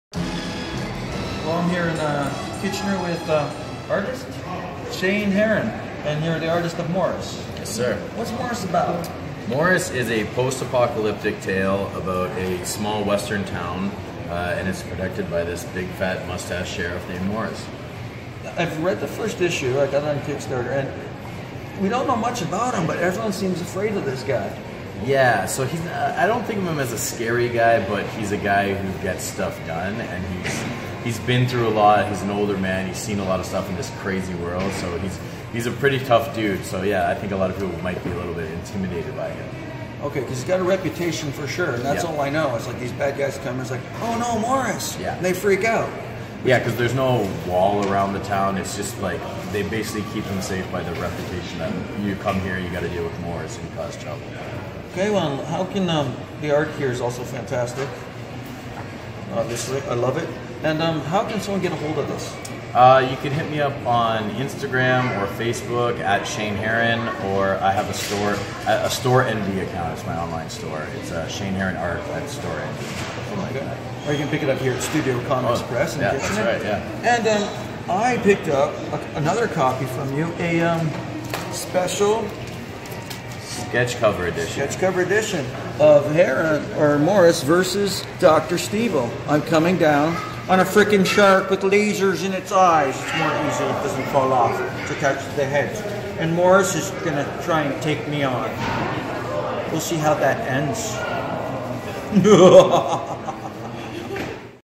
Shane Heron
Dr Stevil chats with Artist, Shane Heron about his new book , Morris